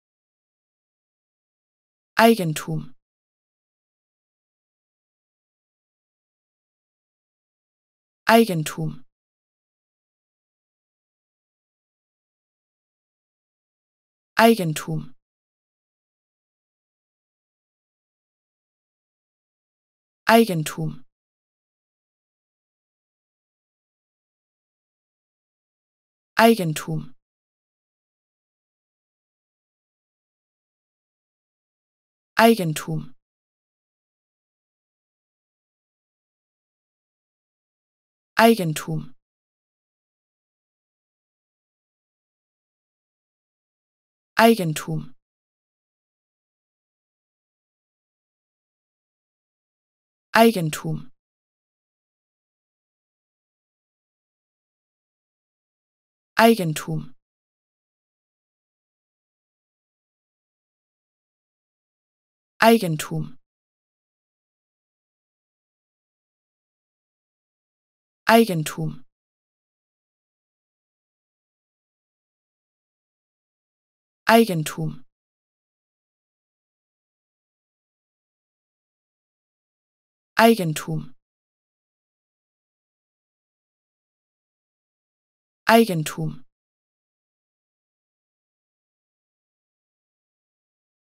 How to Pronounce ''Eigentum'' (Property) Correctly in German
Learn how to say ''Eigentum'' in German with this short pronunciation tutorial.